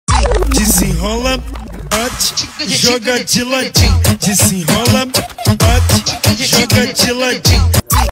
A nossa trend de dancinha por aqui é um pouco diferente… 🎥🎬😛
A equipe segue animada na sua diária 03 da temporada de gravação do 3o episódio da série “O Lugar Antes de Mim”.